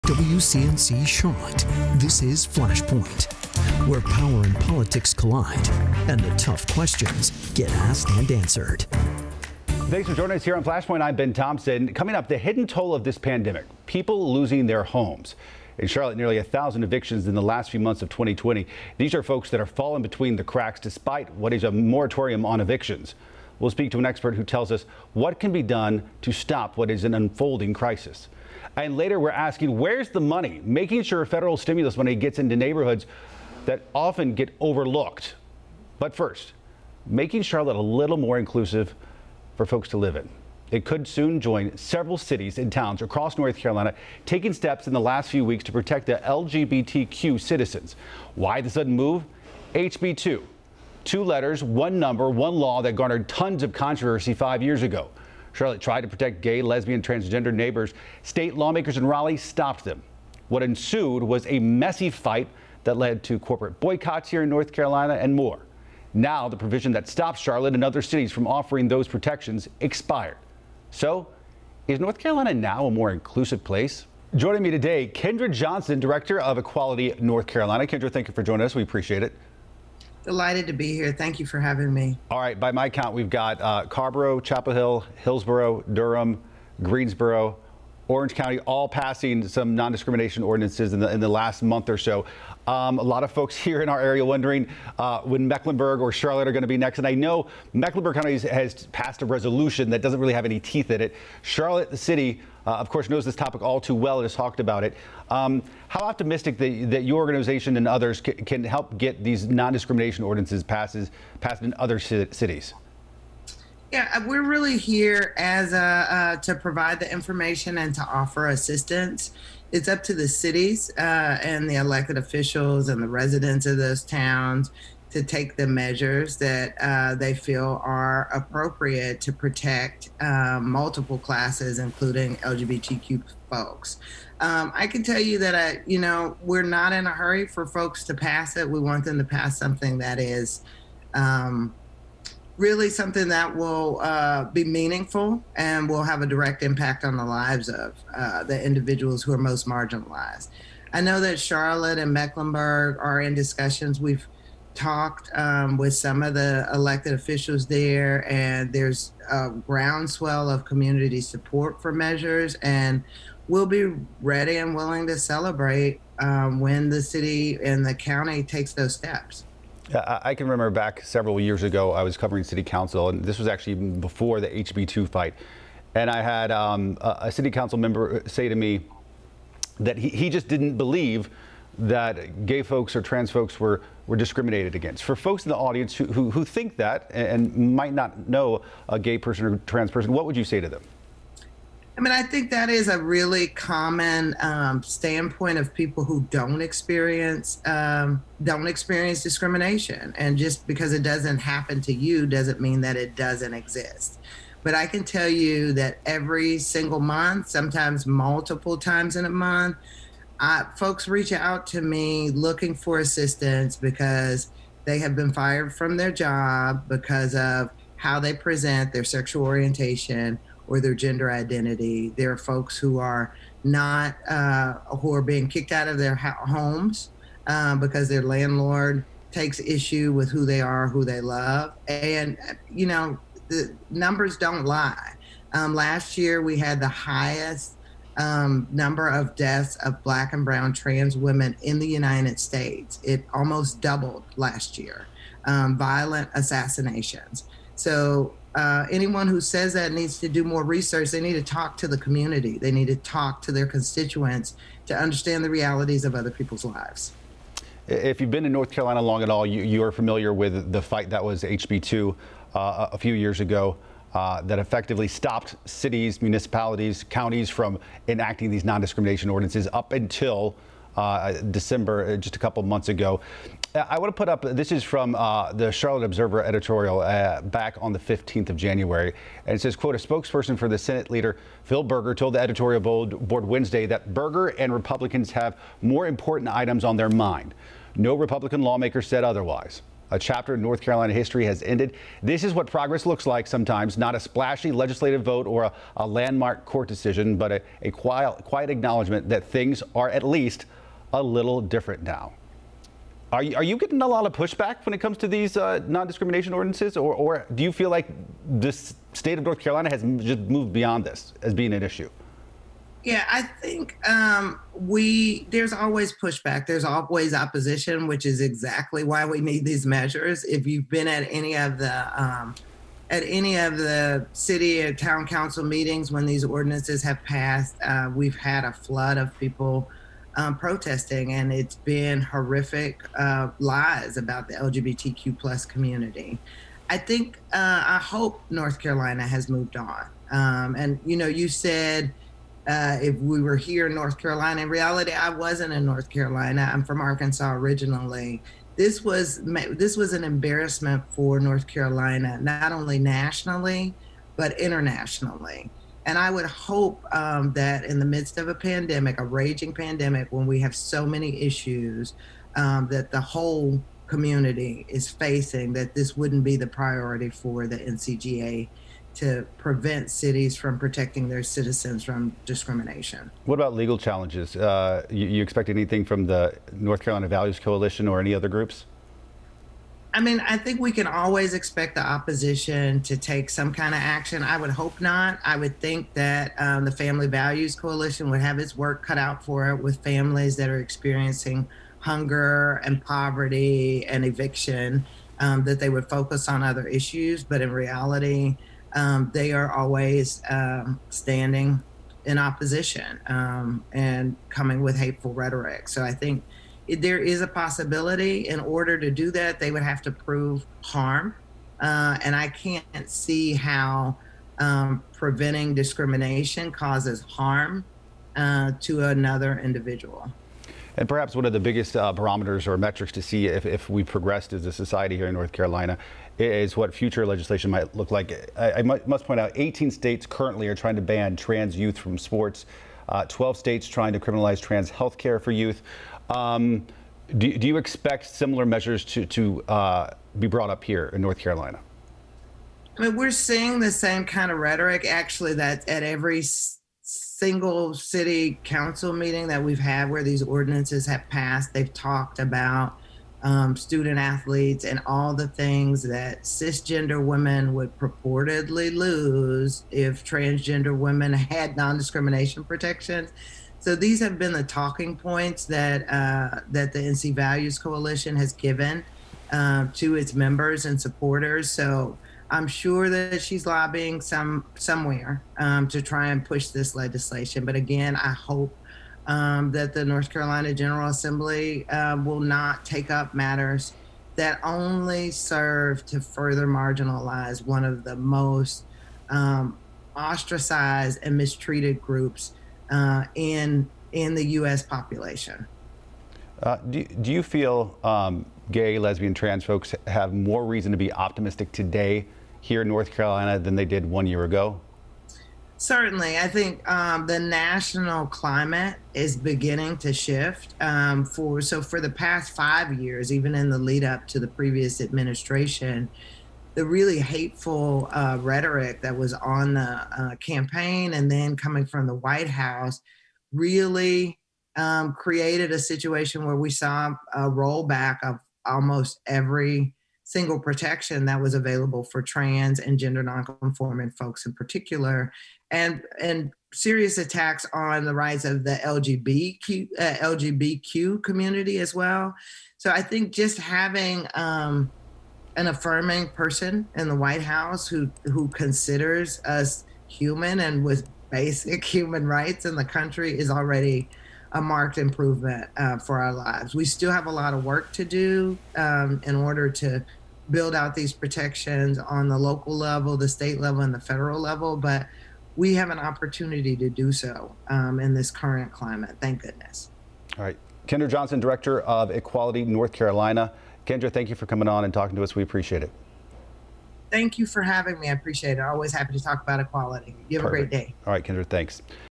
LGBTQ groups see steady progress as more NC towns adopt protections
Several North Carolina cities recently adopted protections for the LGBTQ community. Is Charlotte be next? County leaders have shown a willingness to make it so.